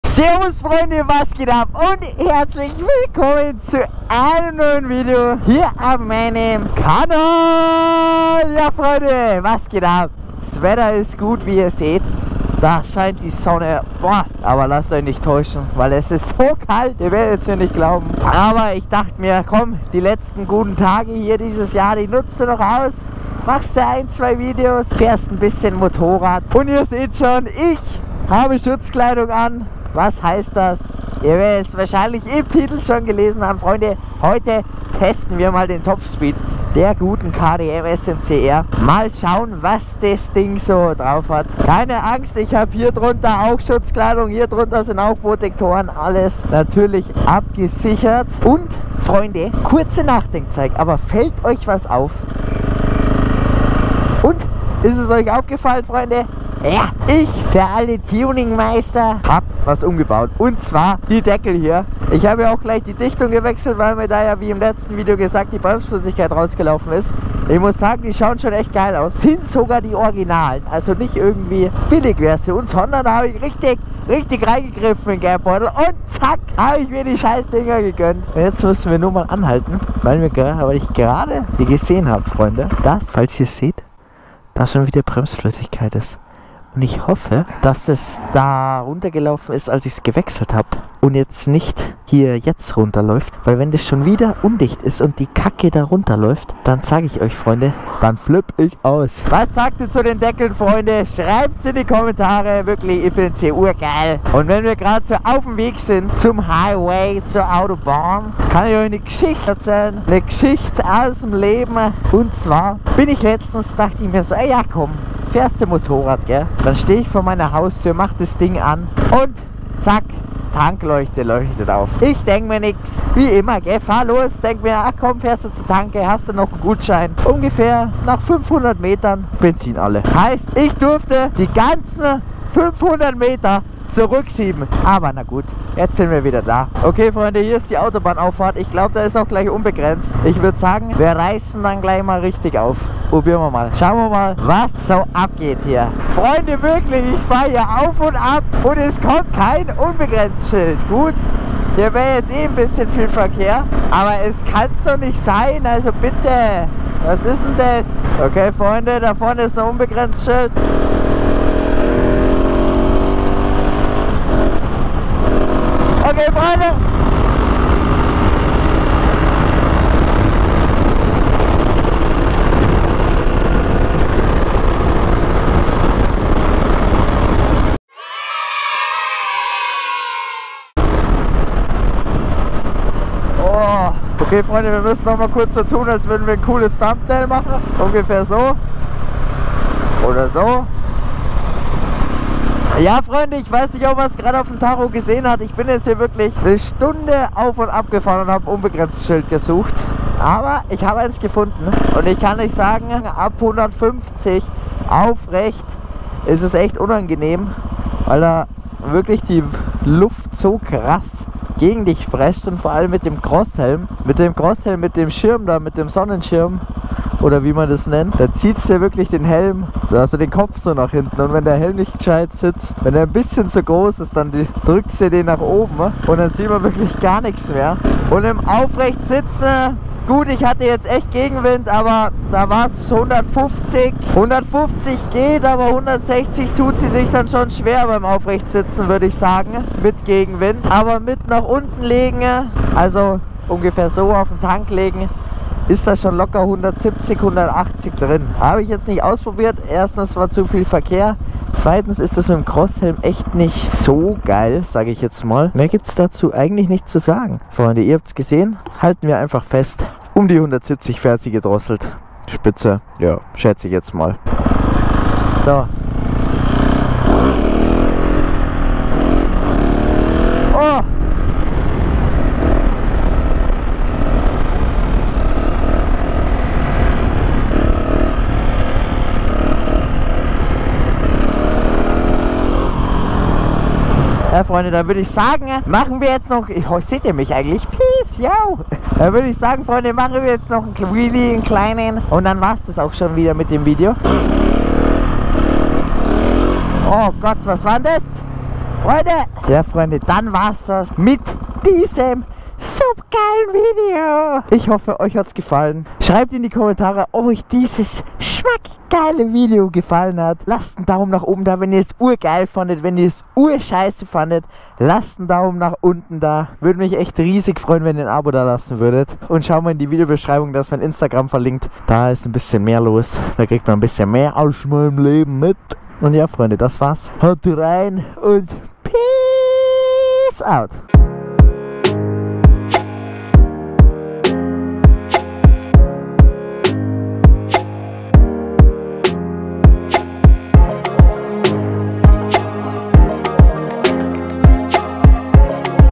Topspeed meiner KTM smcr 690! (A2)
Servus,
Schreib in die Kommentare was du gedacht hättest, wie viel Km/h die KTM schafft.
Keine Ahnung was man hier sonst noch rein schreibt:)